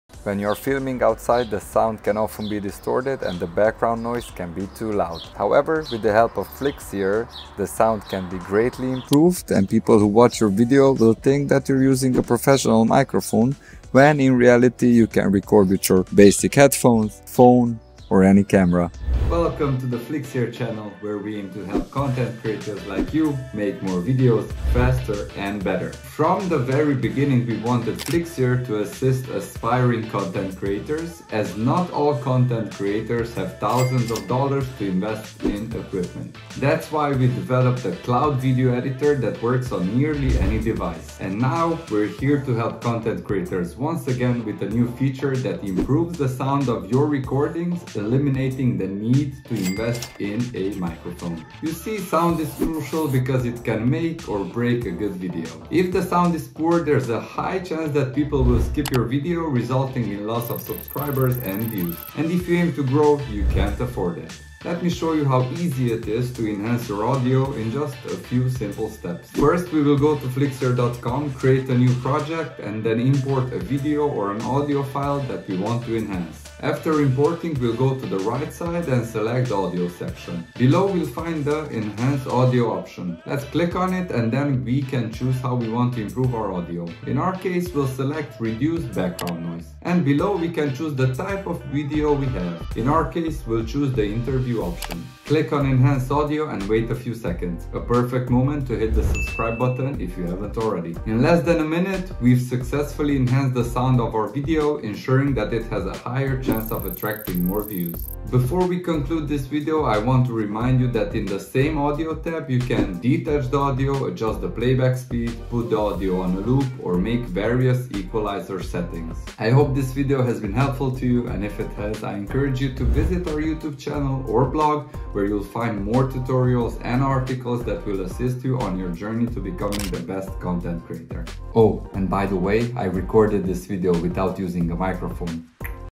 How to improve audio quality online - Step by Step tutorial 2023
Enhance your audio recordings in your web browser with just a few clicks

Need to get better audio for your videos without having to become a professional audio producer? You’re in the right place! Our online audio enhancer tool can automatically make your recordings sound professional with a simple click. It doesn’t matter if you’re making a podcast, trying to improve the sound of a recorded conference, or publishing a lecture. Flixier can improve the quality of your audio, clean up the speech, reduce background noise and fix your loudness and levels. All you need to do is upload your video or audio recording to Flixier, open the audio enhancement menu and let our tool work its magic.

How to use Flixier’s video enhancer tool
1.Upload
Click on Get Started to open Flixier in your web browser. Drag and drop your audio or video files over to the Import menu. If the files aren’t on your computer, you can also bring them over from a number of cloud storage services including Google Drive, Dropbox, or OneDrive.

2.Enhance your audio
After your files are uploaded to your library, double-click on them to add them to the timeline at the bottom of the screen. Select them there, then choose the Enhance Audio option in the right side menu. Choose which enhancements you want our tool to apply and select the type of content you’re making from the dropdown lists, then click on Enhance Audio and give Flixier a few seconds to work its magic.

3.Save your audio or video
When you’re done enhancing your audio, click the Export button in the top right corner of the screen. Choose between the Audio or Video format in the Save As dropdown list, then click on Export and Download. Wait a few minutes for Flixier to process your file and then it will be saved to your computer.